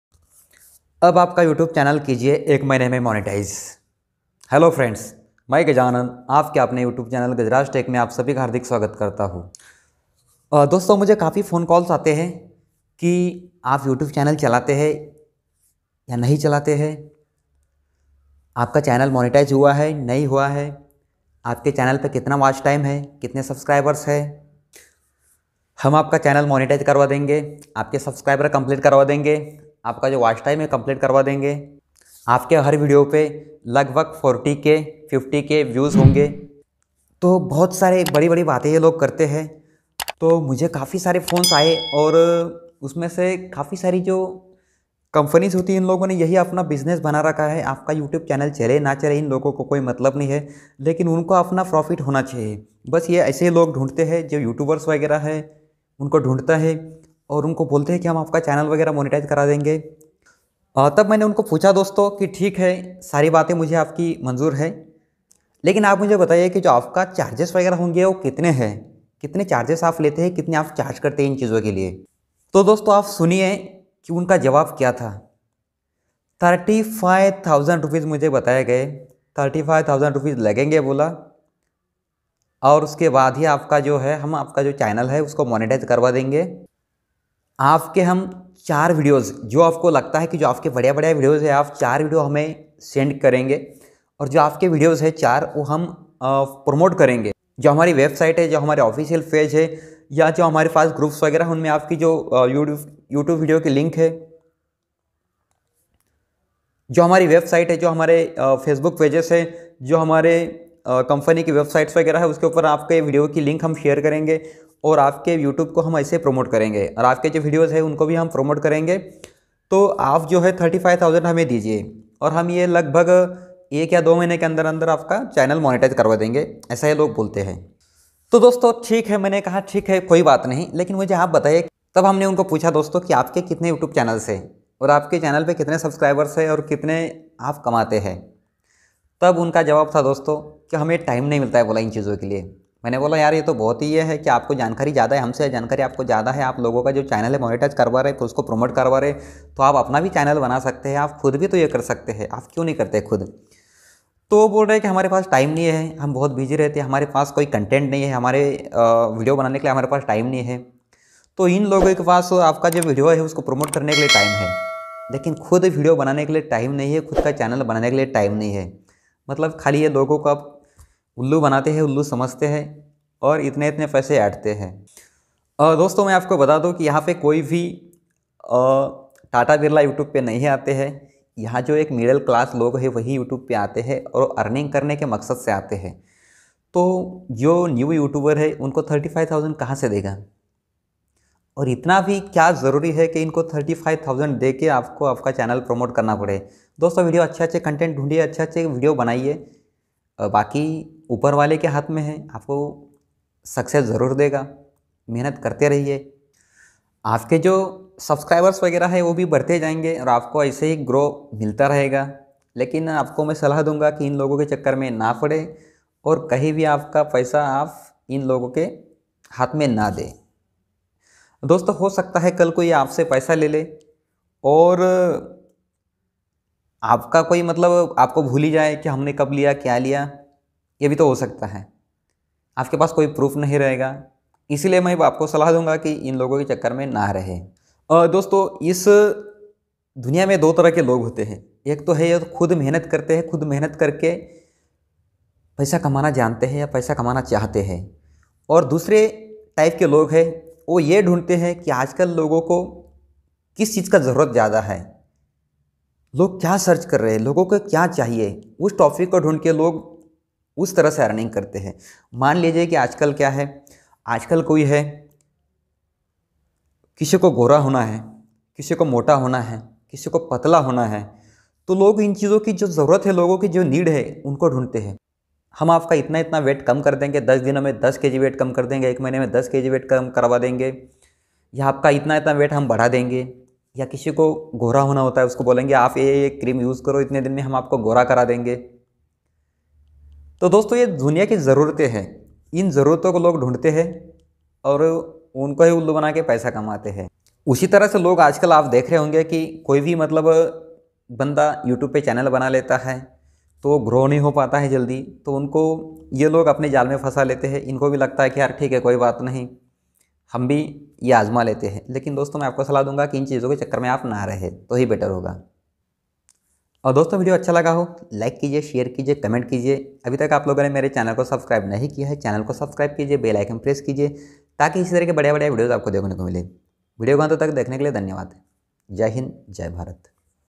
Youtube monetization || how to monetize youtube channel💥💥
hello friends
मैं गजानन आपके आपने  YouTube channel Gajraj tech में आप सभी का हार्दिक स्वागत करता हूं, दोस्तों आज हम बतायेंगे की आपको लोग कैसे  झूठ बोलकर आपसे पैसे लूटते  है की हम आप का youtube channel monetize करवा देंगे बोलकर video अच्छा लगे तो channel subscribe जरूर करें धन्यवाद

SOCIAL MEDIA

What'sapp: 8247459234

youtubechannelverifykaisekartehai2022 youtube channel kaise banaye 2022